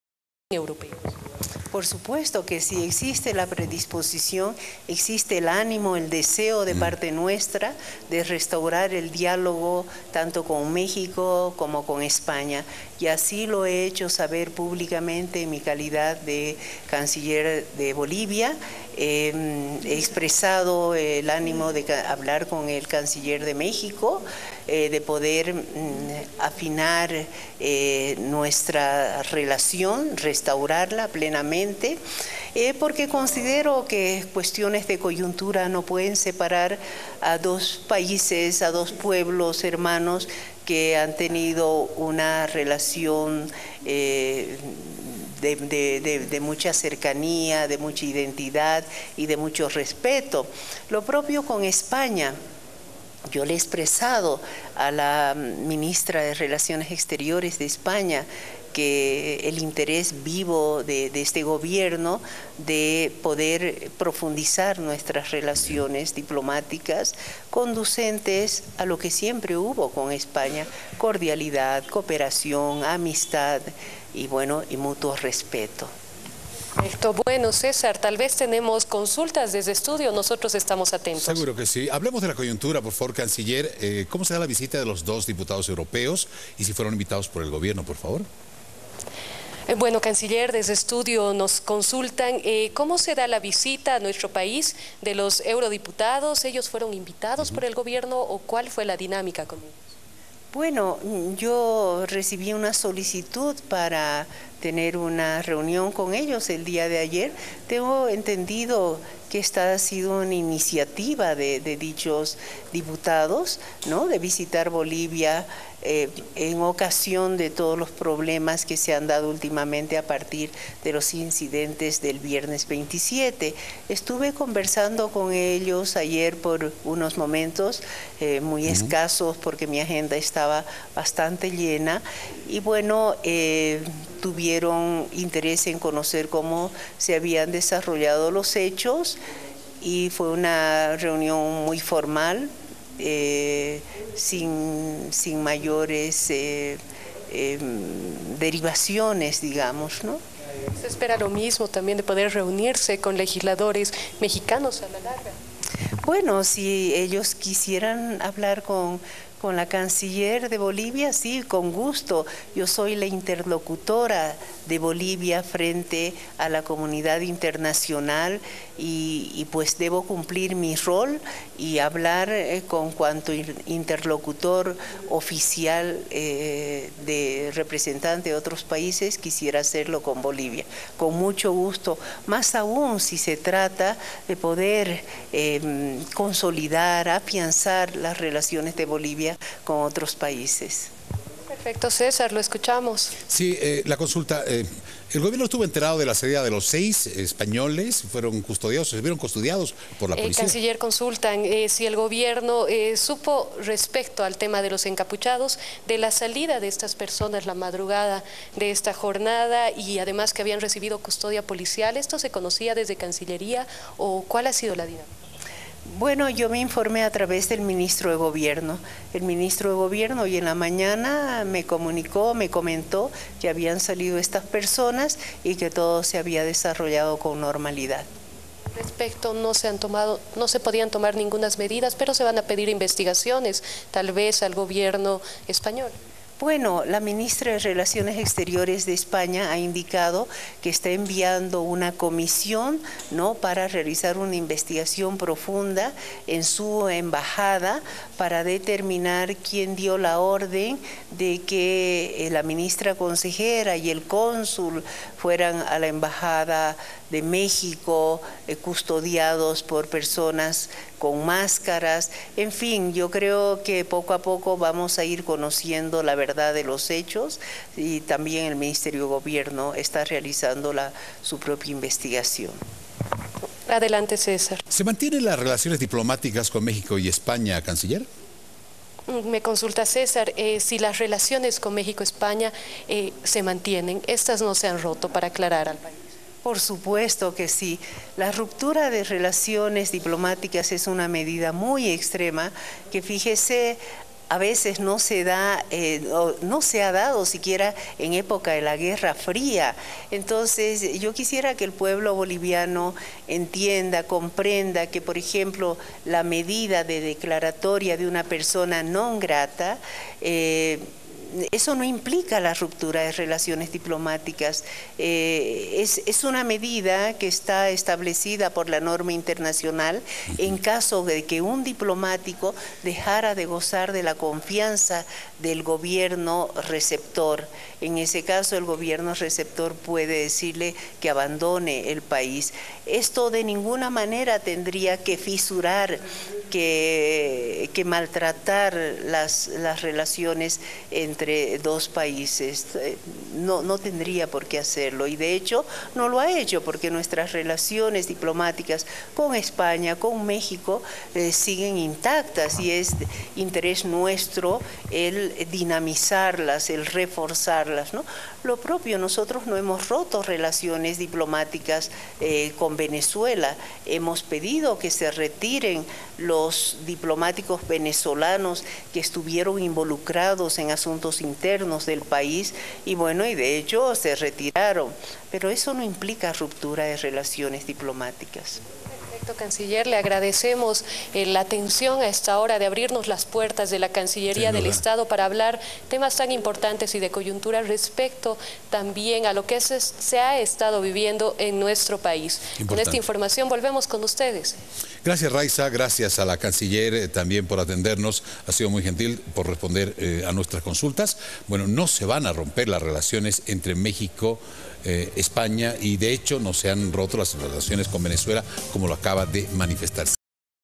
Karen Longaric habla sobre restaurar el Dialogo con Mexico y España
Karen Longaric la canciller Boliviana ahora nos habla de la restauración del dialogo con Mexico y ESpaña